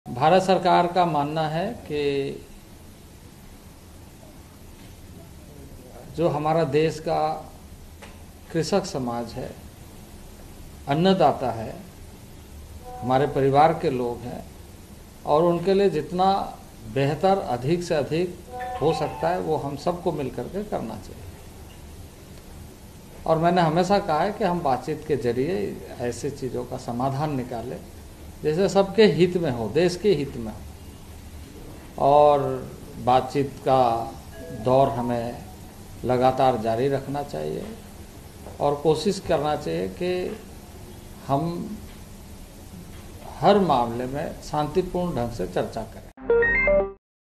Farmers protest : देश के किसान हमारे परिवार जैसे, समाधान निकलने तक होगी वार्ता- कृषि मंत्री
Farmers protests: केन्द्रीय कृषि मंत्री अर्जुन मुंडा ने कहा है कि देश के किसान हमारे परिवार हैं और उनसे समाधान निकलने तक वार्ता जारी रहेगी. किसानों के साथ तीन बार वार्ता सफल नहीं होने के बाद उन्होने ये बयान दिया है